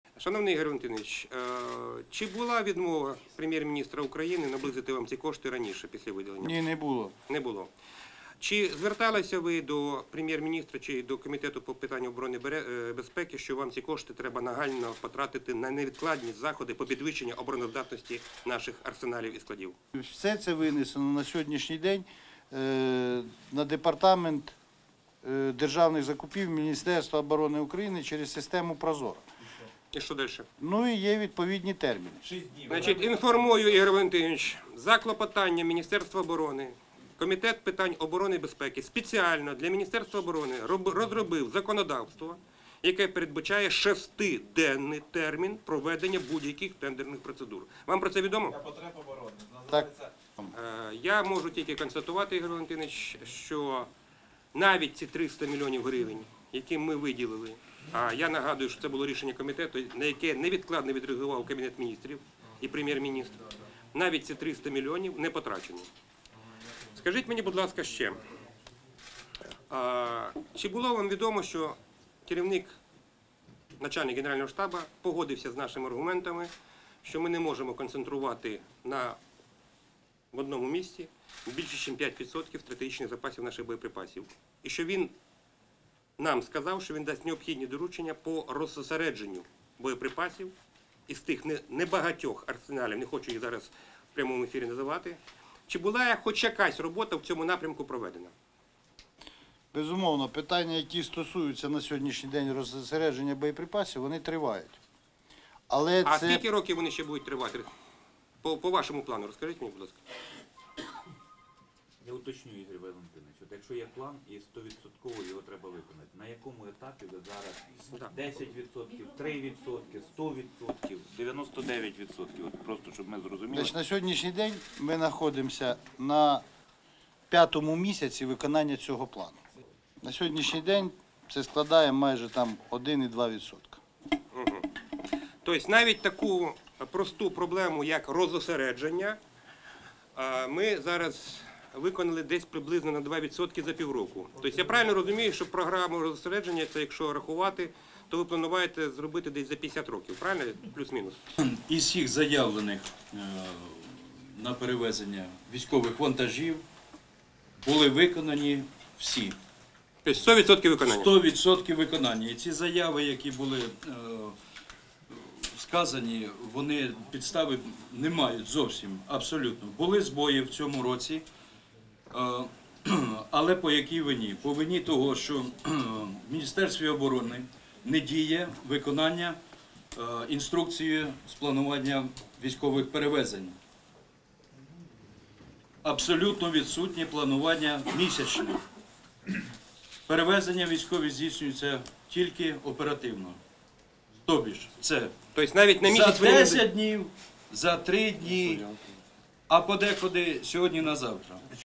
Міноборони виконало роботи по розосередженню боєприпасів лише на рівні 1%
Роботи по розосередженню боєприпасів на військових складах виконані на рівні 1%, а виділені Урядом кошти, спрямовані на підвищення їх безпеки, Міністерство оборони не змогло освоїти через бюрократичні процедури.

Про це йшлося на засіданні Комітету з питань національної безпеки і оборони щодо причин вибухів у Калинівці та ситуації із безпекою на українських військових складах.

"Чи зверталися ви до Кабінету Міністрів або комітету, що вам ці кошти треба нагально витратити на невідкладні заходи для підвищення безпеки наших військових складів?", - запитав голова профільного комітету Сергій Пашинський у присутнього на засіданні заступника міністра оборони, генерал-лейтенанта Ігоря Павловського.

"Все це винесено на сьогодні на департамент державних закупівель Міноборони через систему ProZorro і є певні терміни", - відповів Павловський.

"Інформую, що за клопотанням Міністерства оборони комітет спеціально для нього розробив законопроект, який передбачає шестиденний термін для відповідних закупок на потреби оборони", - зауважив голова комітету Сергій Пашинський.

На низькому рівні виконання також знаходяться й роботи по розосередженню боєприпасів.

"Питання, які стосуються розосередження боєприпасів, вони тривають", - пояснив заступник міністра оборони Павловський.

"Я уточню, якщо є план і його на 100% треба виконати, на якому етапі це зараз? 10%, 3%, 100%, 99%, щоб ми розуміли", - звернувся до представника Міністерства оборони народний депутат фракції "Народний фронт", член комітету Юрій Береза.

"На сьогодні ми знаходимося на п'ятому місяці виконання цього плану. На сьогоднішній день це складає 1,2%", - сказав заступник міністра оборони Ігор Павловський.

"Тобто, навіть таку проблему, як розосередження, ми виконали десь на 2%. Якщо порахувати, то ви плануєте все це зробити за 50 років", - зауважив Пашинський.

Присутній на засіданні представник "Укрзалізниці" зі спецперевезень уточнив, що усі заявки на виконання військових вантажів були виконані попри інсинуації начальника Генерального штабу Віктора Муженка.

"На 100% виконання. Ці заяви, які були сказані, вони підстав не мають. Були збої в цьому році, але з якої вини? В Міністерстві оборони не діє виконання інструкції з планування військових перевезень. Абсолютно відсутнє місячне планування перевезень. Вони здійснюються тільки оперативно за 10 днів, за три дні, а інколи "із сьогодні на завтра", - пояснив представник "Укрзалізниці".

Приєднуйся!